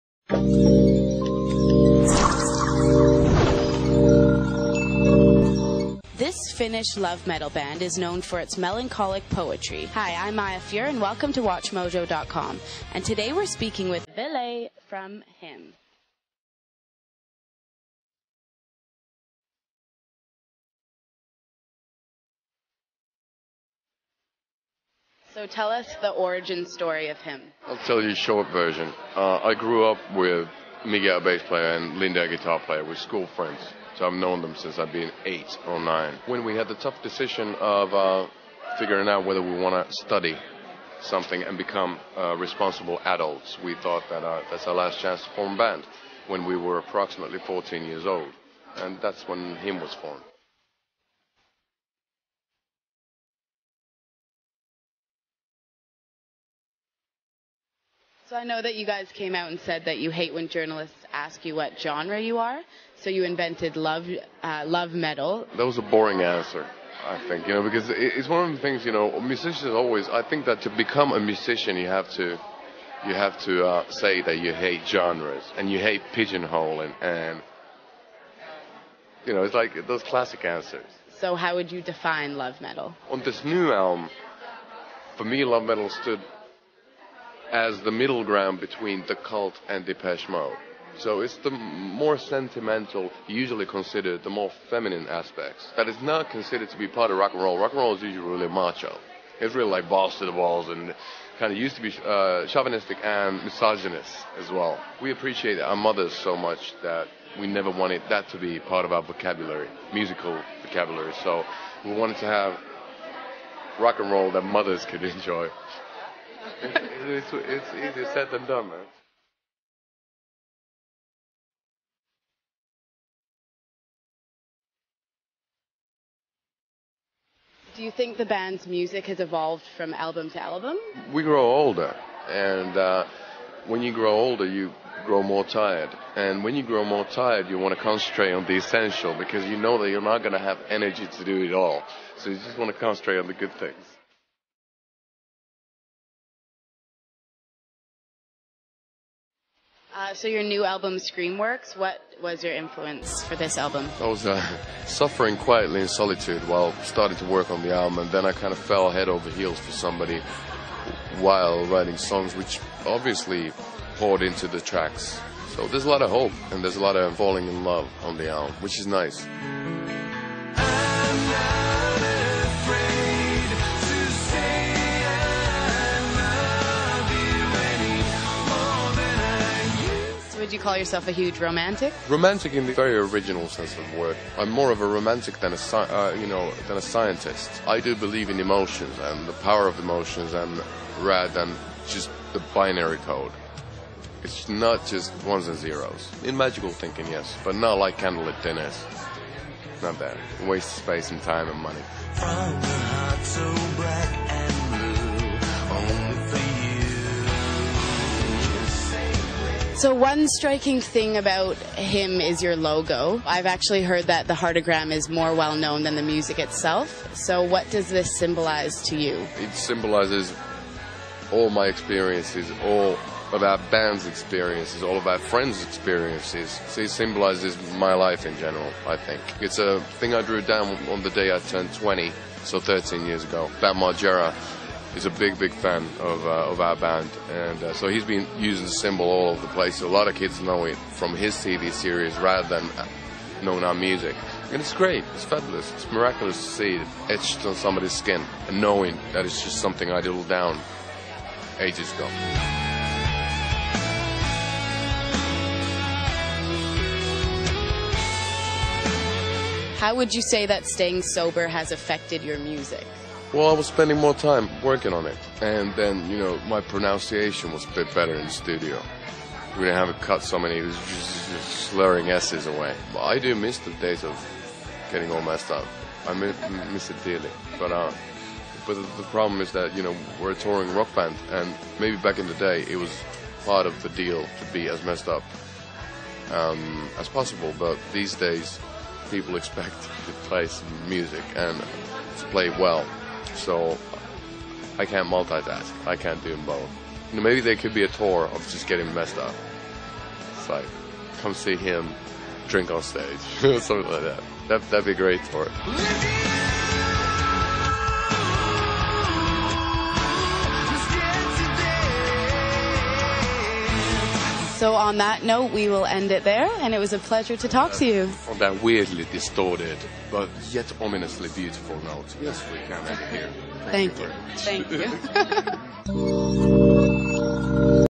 Entrevista Watchmojo.com con Ville Valo de HIM (Subtitulada)
Hola chicos,bueno aquí les traigo esta entrevista que tiene las ''mismas preguntas de siempre'' pero lo interesante son las respuestas de Ville :) disfrútenla
Únete a: